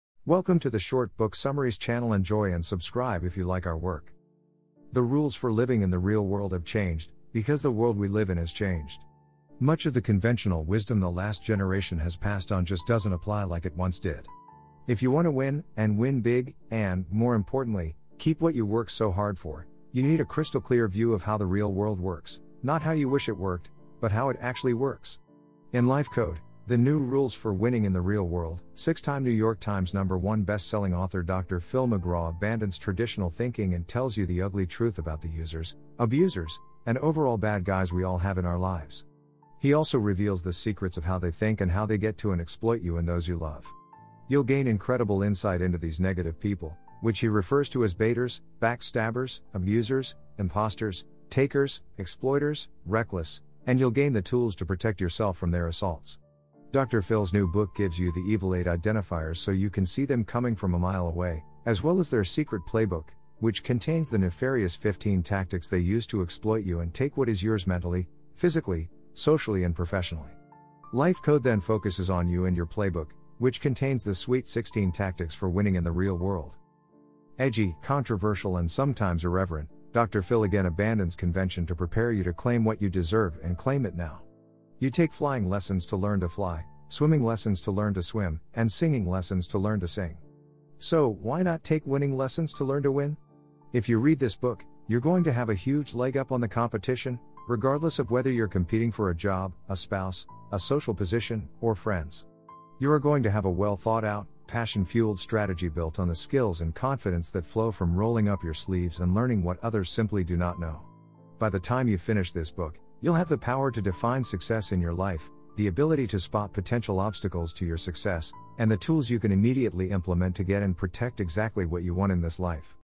Short Book Summary of Life Code The New Rules For Winning in the Real World by Phillip C McGraw
The rules for living in the real world have changed, because the world we live in has changed. Much of the conventional wisdom the last generation has passed on just doesn’t apply like it once did. If you want to win, and win big, and, more importantly, keep what you work so hard for, you need a crystal-clear view of how the real world works — not how you wish it worked, but how it actually works.
In Life Code: The New Rules for Winning in the Real World, six-time New York Times #1 best-selling author Dr. Phil McGraw abandons traditional thinking and tells you the ugly truth about the users, abusers, and overall “bad guys” we all have in our lives. He also reveals the secrets of how they think and how they get to and exploit you and those you love. You’ll gain incredible insight into these negative people, which he refers to as BAITERs (Backstabbers, Abusers, Imposters, Takers, Exploiters, Reckless), and you’ll gain the tools to protect yourself from their assaults. Dr. Phil's new book gives you the “Evil Eight” identifiers so you can see them coming from a mile away, as well as their “Secret Playbook,” which contains the “Nefarious 15” tactics they use to exploit you and take what is yours mentally, physically, socially and professionally. Life Code then focuses on you and your playbook, which contains the “Sweet 16” tactics for winning in the real world.

Edgy, controversial and sometimes irreverent, Dr. Phil again abandons convention to prepare you to claim what you deserve and claim it now. You take flying lessons to learn to fly, swimming lessons to learn to swim, and singing lessons to learn to sing. So, why not take winning lessons to learn to win?
If you read this book, you’re going to have a huge leg up on the competition, regardless of whether you’re competing for a job, a spouse, a social position, or friends. You are going to have a well-thought-out, passion-fueled strategy built on the skills and confidence that flow from rolling up your sleeves and learning what others simply do not know. By the time you finish this book, you’ll have the power to define success in your life, the ability to spot potential obstacles to your success, and the tools you can immediately implement to get and protect exactly what you want in this life.